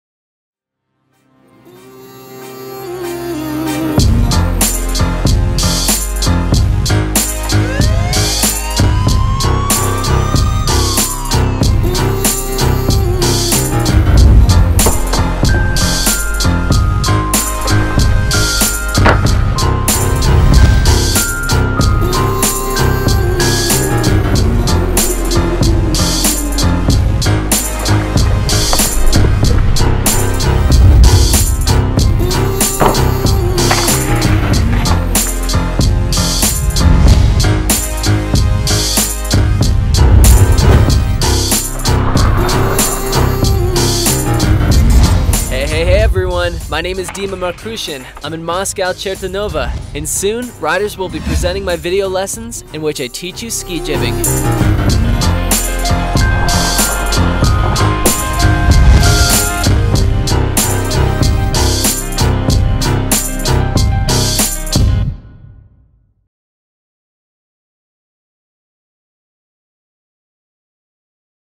Ski by Riders
– Join 400,000 riders from all corners of the world to learn skis tricks and find riders nearby!

We are proud to announce a new ski jibbing section on the RIDERS app in collaboration with Life Steeze Media and Dima Makrushin. Winter is coming straight in your phone. After meeting with the RIDERS app you'll be surprised what you can do on your skis.